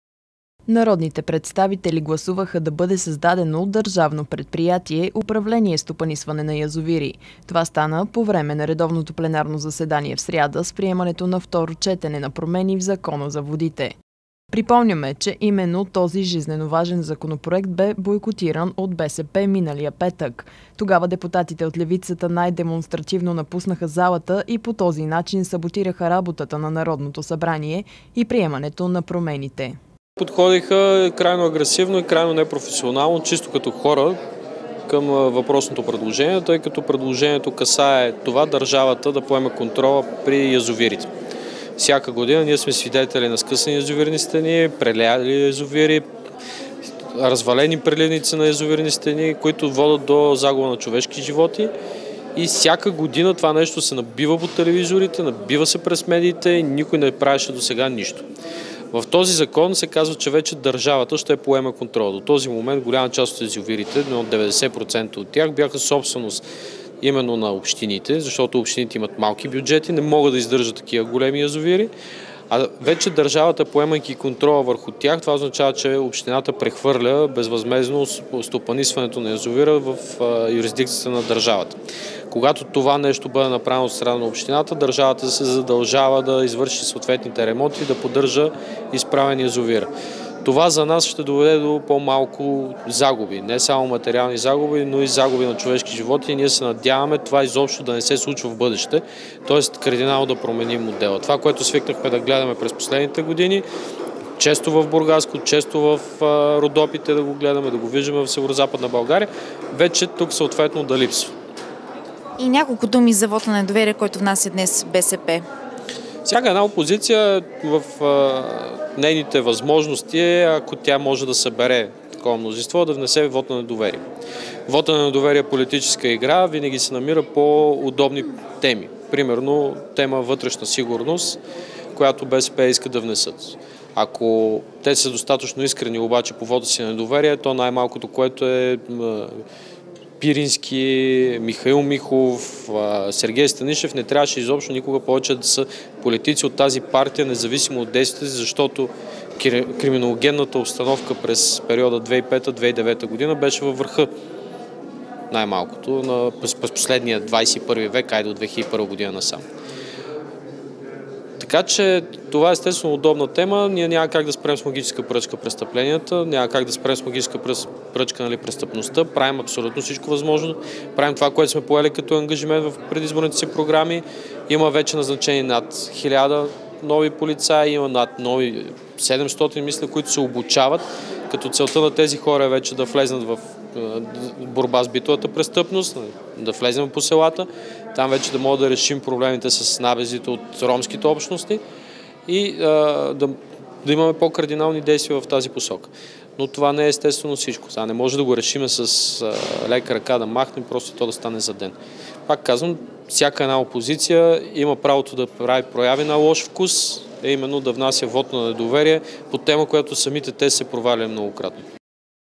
Държавата поема стопанисването на язовирите /20.06.2018 г./
Народните представители гласуваха да бъде създадено Държавно предприятие "Управление и стопанисване на язовири". Това стана по време на редовното пленарно заседание в сряда с приемането на второ четене на промени в Закона за водите. Припомняме, че именно този жизненоважен законопроект бе бойкотиран от БСП миналия петък